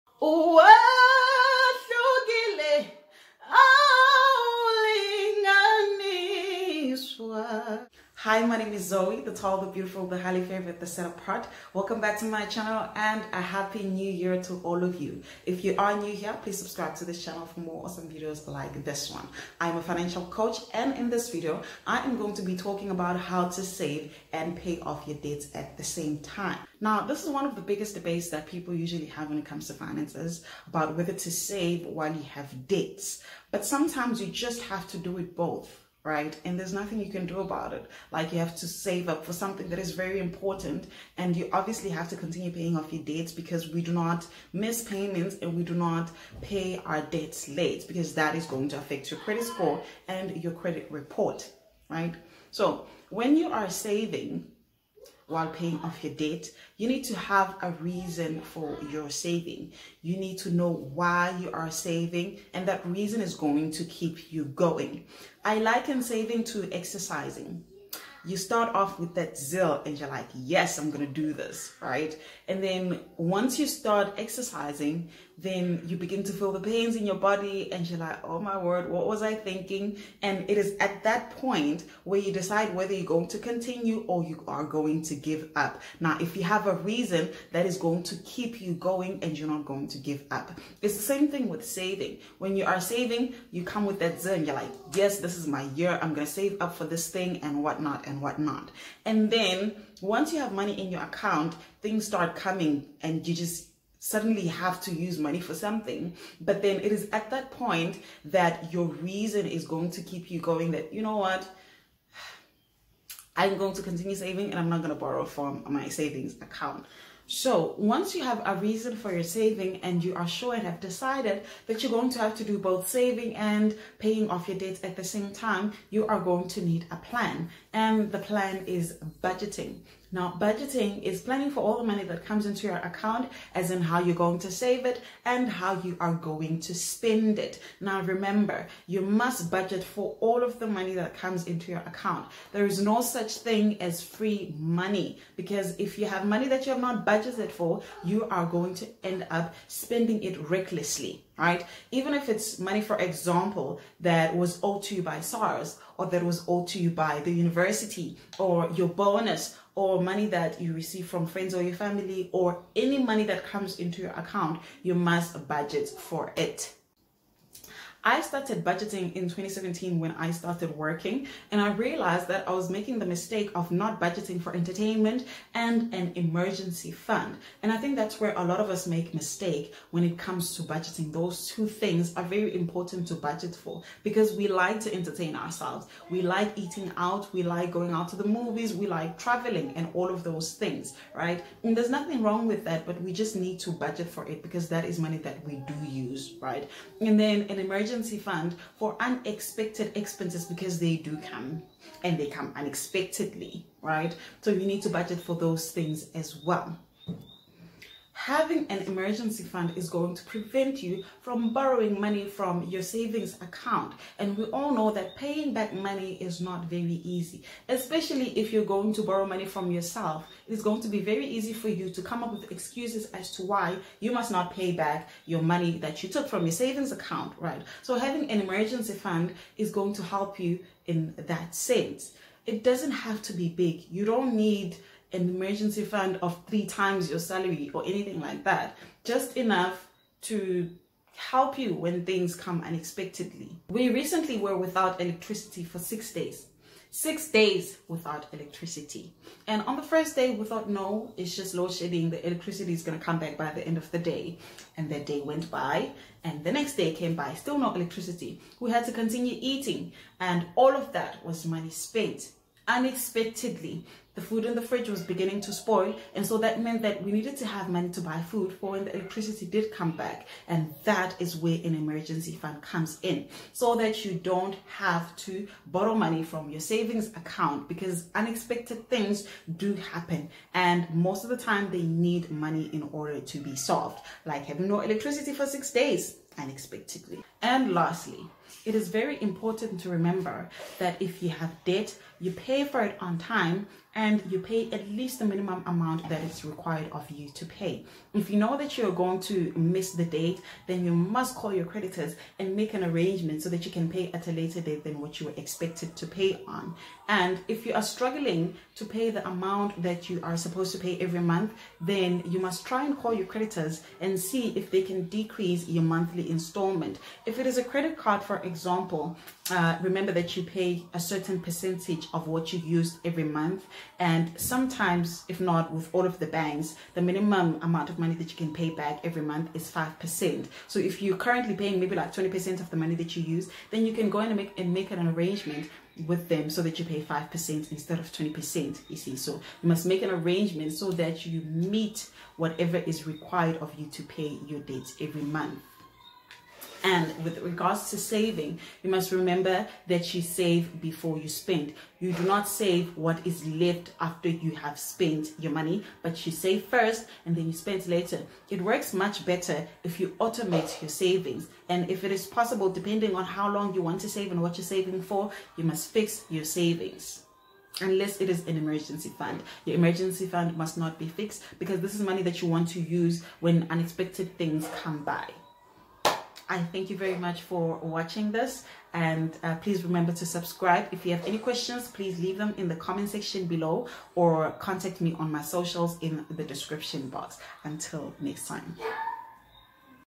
HOW TO SAVE AND PAY FOR DEBTS AT THE SAME TIME|ADULTING AND FINANCES|SA YOUTUBER
The new year is here,its time to manage your finances better. I'm kicking the year by sharing with you how to save and pay for debts at the same time because sometimes you just gotta do both.

you can use Tubebuddy to grow your YouTube channel. click on the following link: